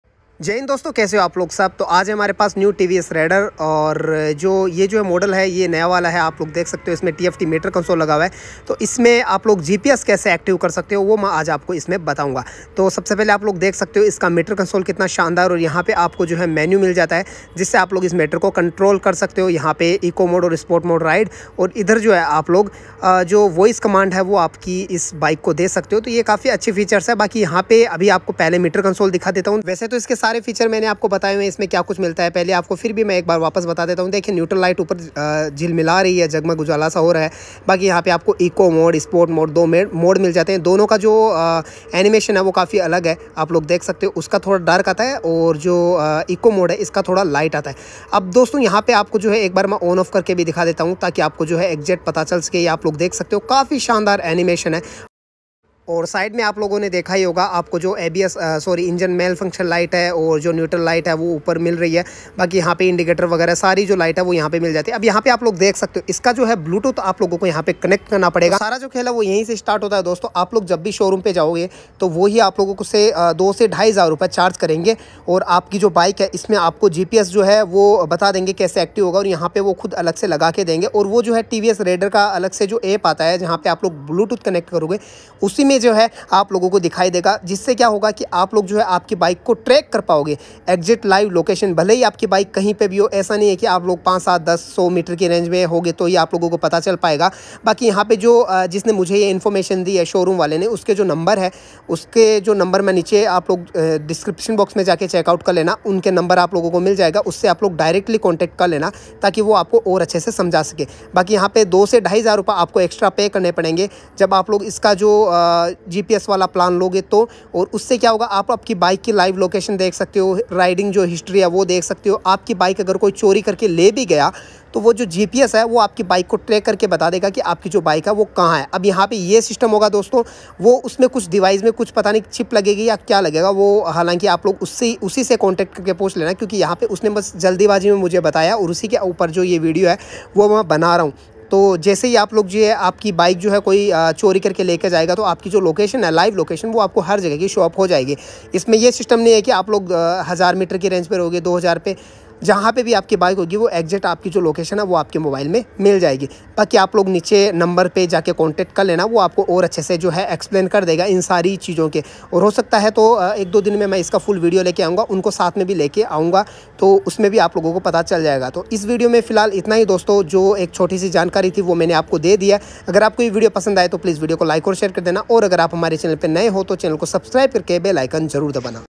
2023 Tvs Raider New Model Meter Console | GPS कैसे ऐक्टिवट् करे ..?
2023 Tvs Raider New Model Meter Console | GPS कैसे ऐक्टिवट् करे ..?

Dosto is video mai maine aapko raider 125 ke model mai GPS Kaise active kar skte ho iske baare mai bataya hai,
Hope ki aapko ya jankari achi lagi.